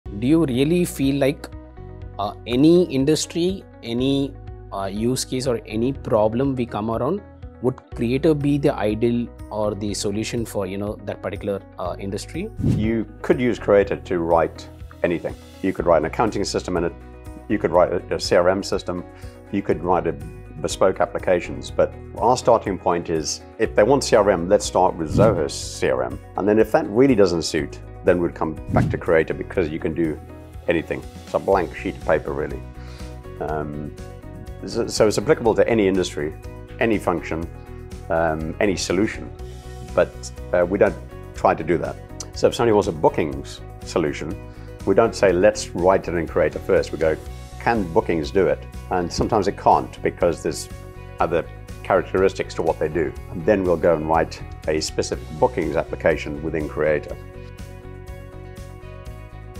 Would low-code platforms such as Zoho Creator be well-suited for any industry or challenge?
To sign up for a 15 day trial and explore the digital transformation of your business,